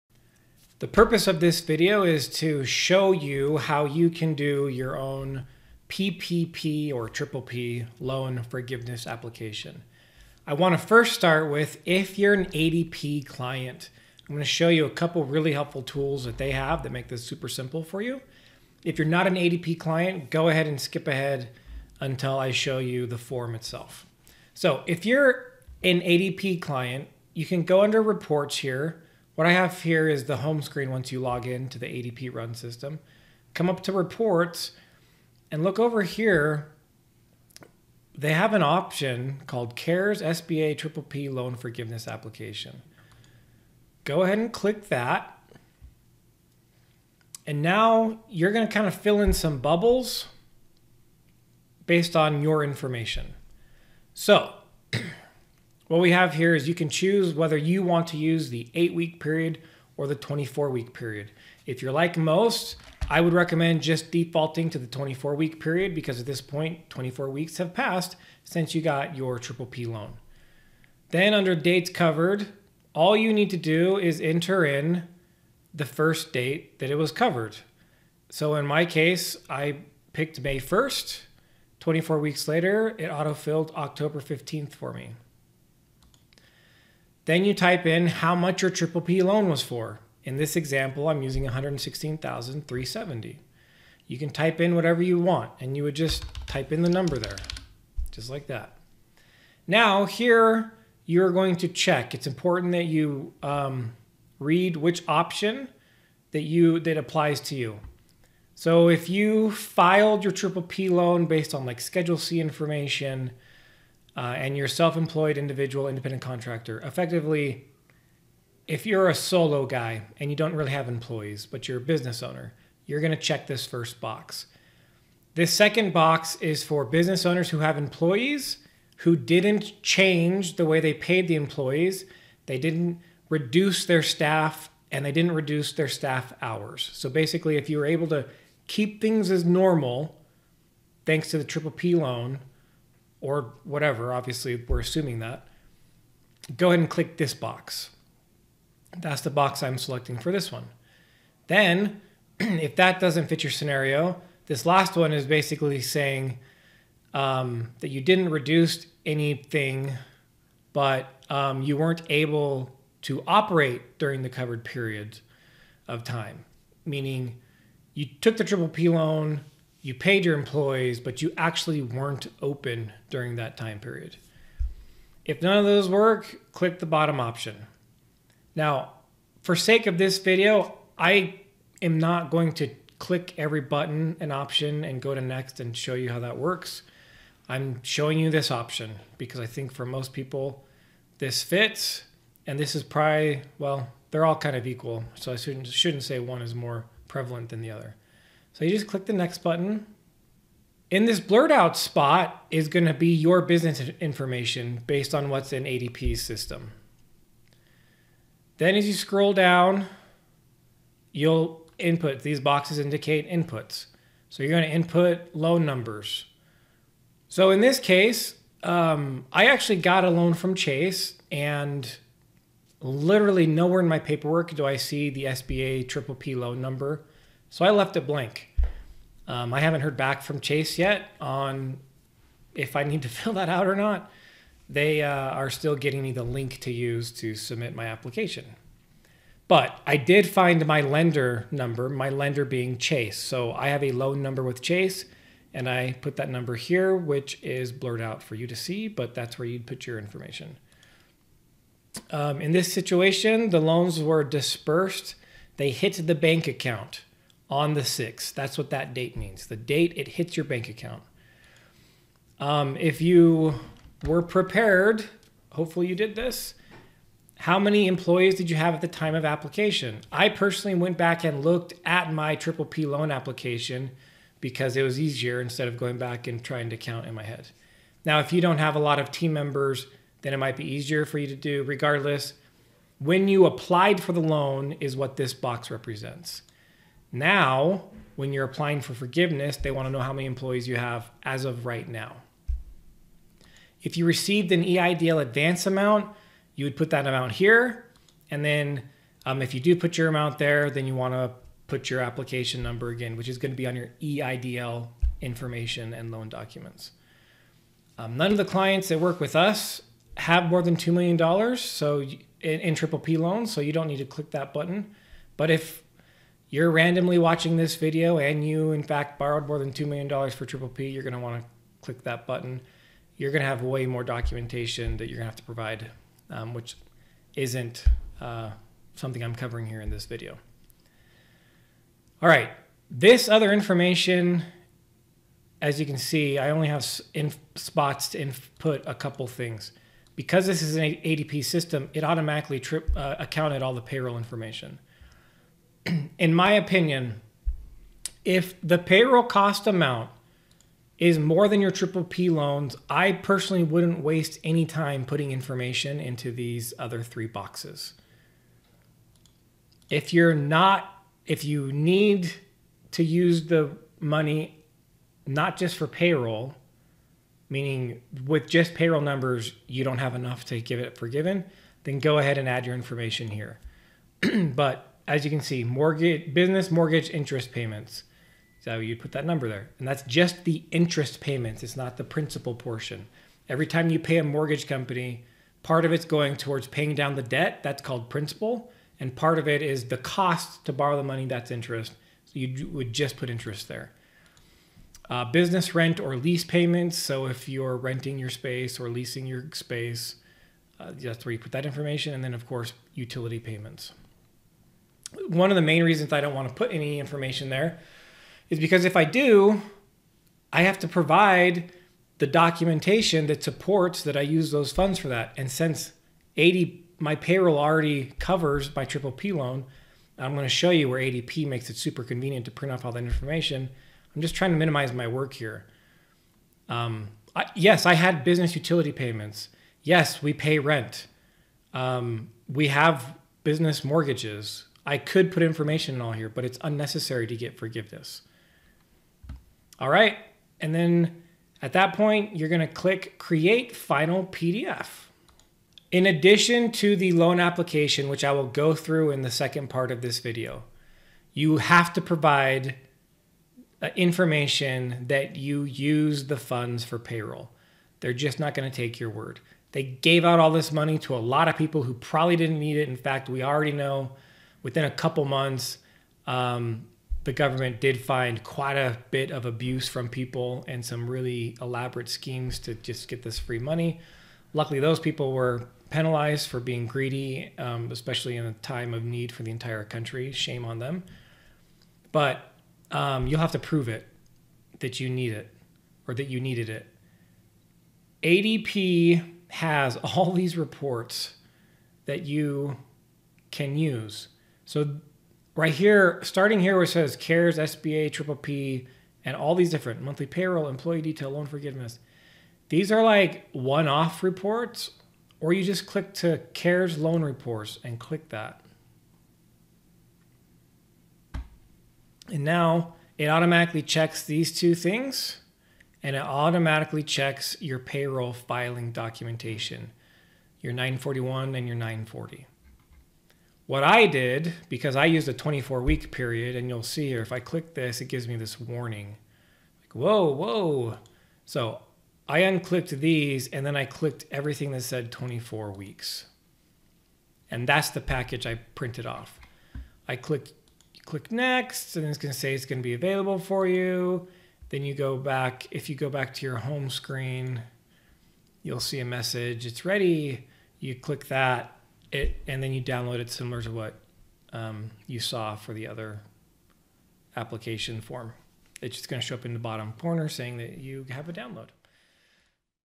PPP Forgiveness APP - ADP Resources | Incite Tax
If you use ADP for your payroll services this video will provide you the exact steps to take and where to find the information you will need to quickly fill out your triple P loan forgiveness application. The next video will the actual walkthrough of the main application.

Click this link for more FREE resources to GROW your WEALTH